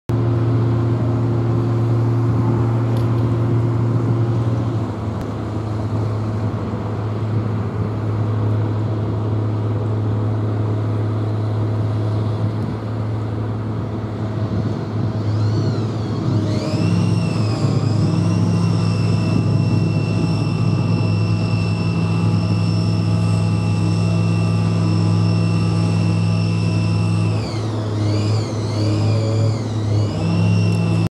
‘She Came from the Stars’ by Vexta (on Manchester St.) (2016-06-02-15:34)
Zooming out from the view of the mural 'She Came from the Stars' by Vexta, from Manchester St. Recorded 2016, 2nd of June at 3:34pm.